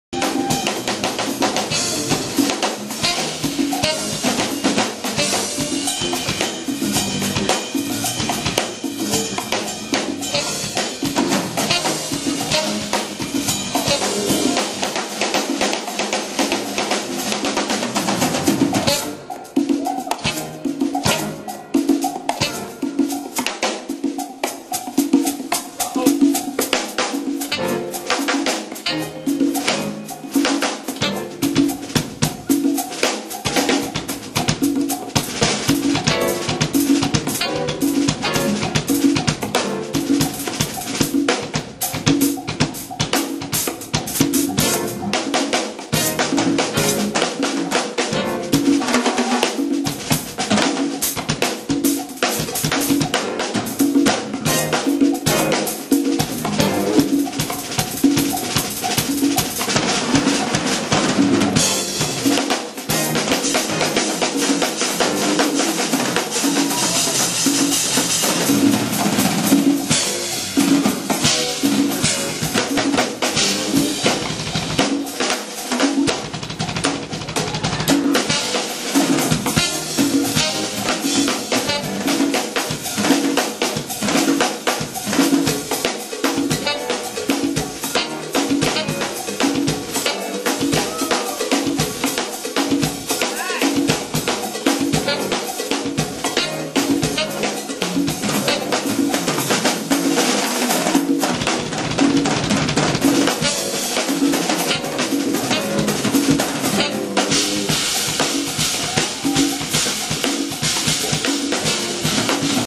Sonny Emory Hypnofunk Drumsolo 2008
State of the Art performing at The Sonny Emory Drumcamp June 2008.

!! Sorry for the short video. My camerabattery failed on me !!